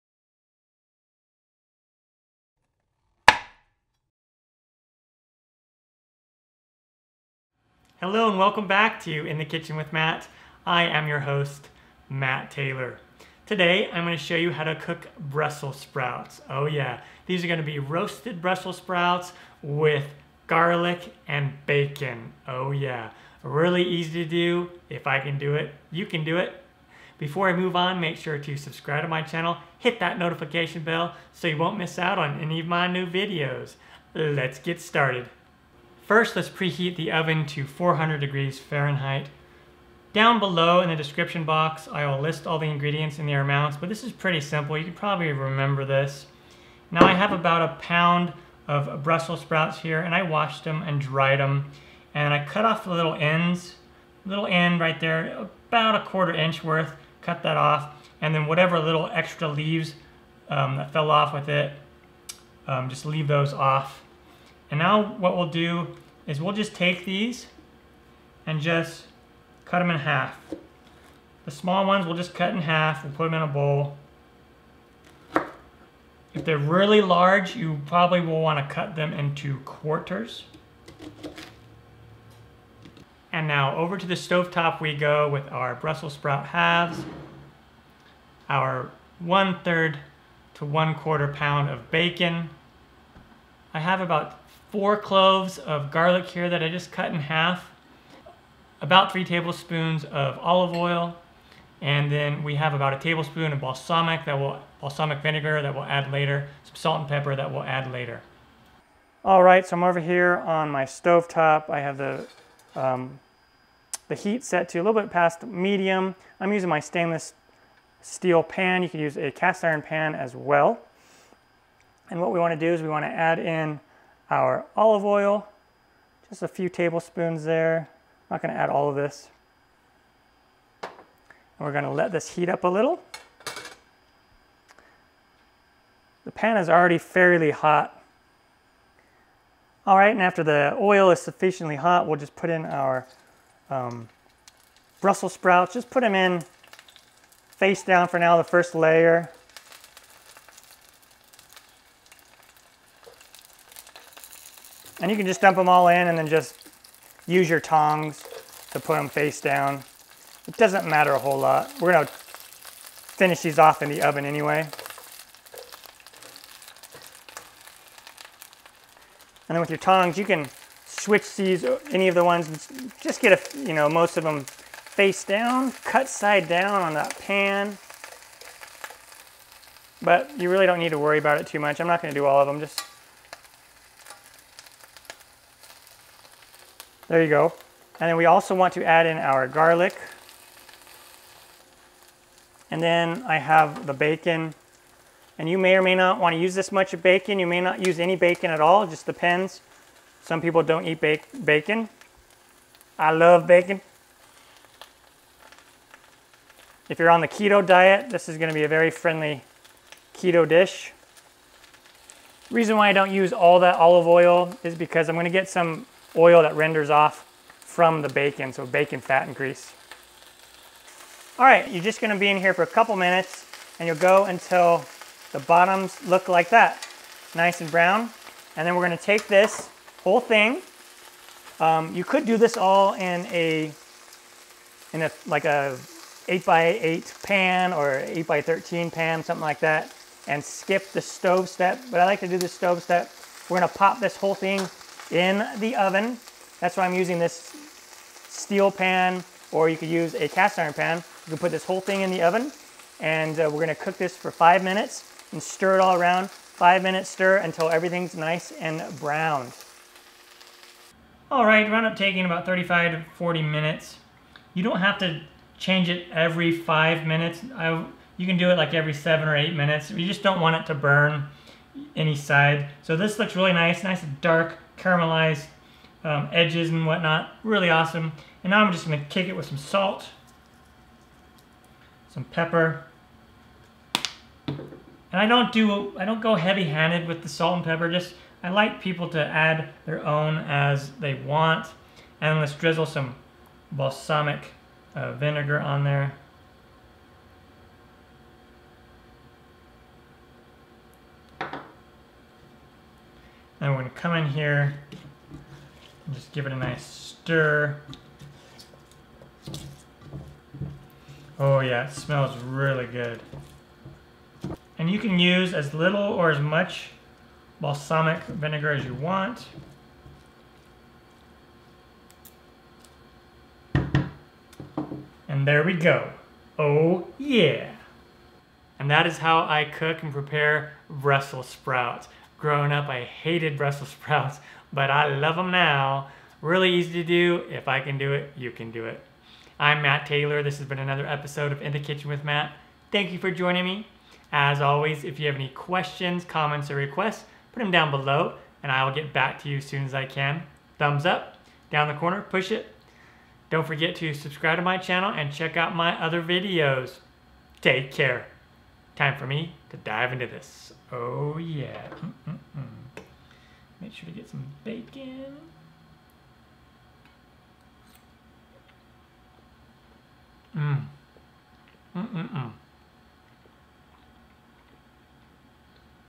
How to Cook Brussel Sprouts | Roasted Garlic and Bacon Brussel Sprouts Recipe
Or buy the kindle version here on Amazon

In this episode of In the Kitchen with Matt I will show you how to cook Brussel Sprouts. I love this recipe for Roasted Brussel Sprouts with garlic and bacon. Growing up I did not like eating Brussel Sprouts, but now I love them! They are one of my favorite roasted veggies to eat. And it is a great recipe if you happen to be on the Keto diet. They are very easy to make, if I can do it you can do it, let's get started!

What is the difference between success and failure on the Keto diet?

Ingredients:
About 1 pound of Brussel Sprouts, washed. (450g)
2 to 3 Tbsp. of olive oil (30 to 45ml)
4 to 6 garlic gloves, halved
3 to 4 strips of bacon (certainly add more or less or leave it out)
1 Tbsp. Balsamic vinegar (15ml)
Garlic powder (to taste, not shown in the video, but I like adding it)

Tools:
Cast iron or stainless steel skillet (oven safe) or use a 9x13 pan and skip the stovetop step
Tongs
Knife
Cutting board

Video Production Gear I use:
OBS Studio